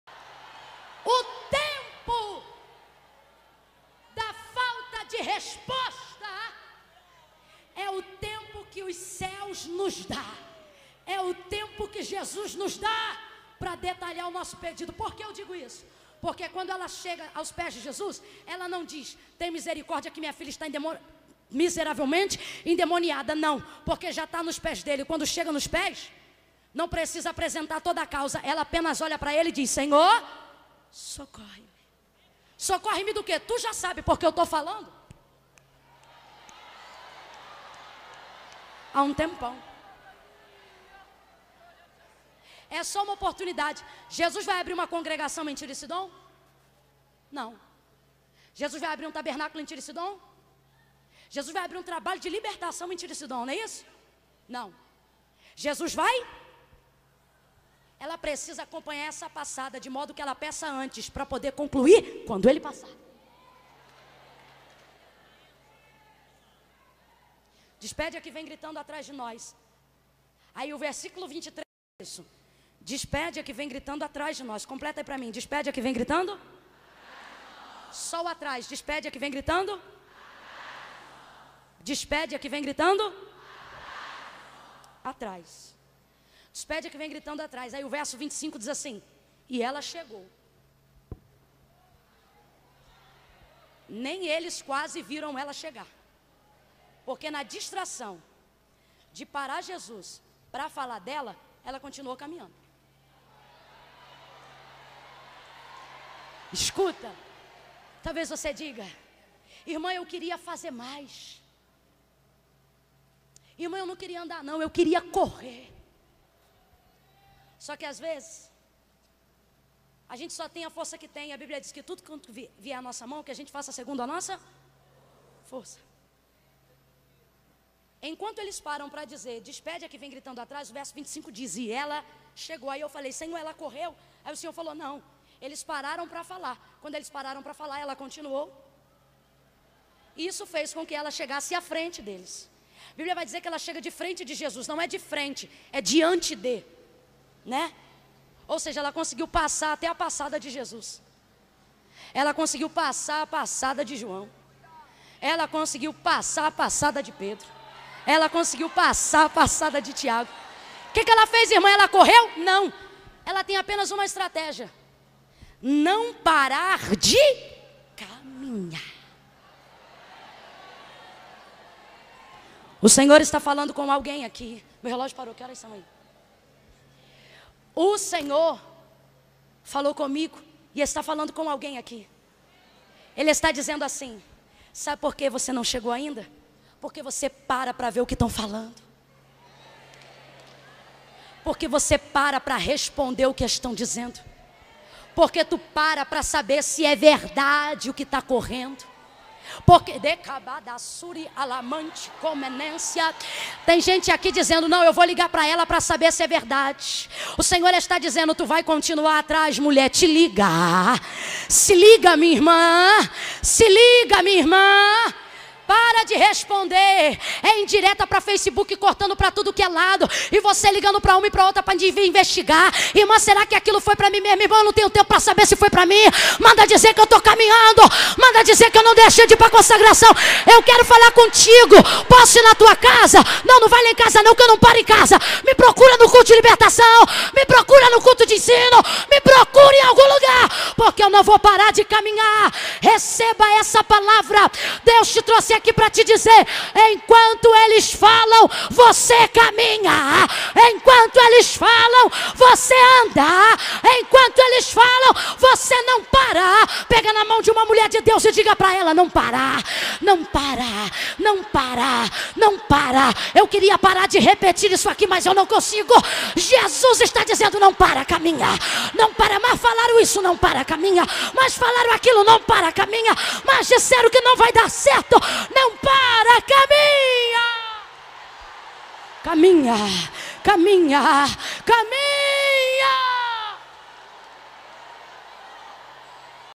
Pregação Curta e impactante, Camila Barros
Através de uma pregação curta  Deus pode nos surpreender e tocar nosso coração.
Aqui podemo ver um grande exemplo de pregação, onde através de poucas palavras Deus revela grandes coisas.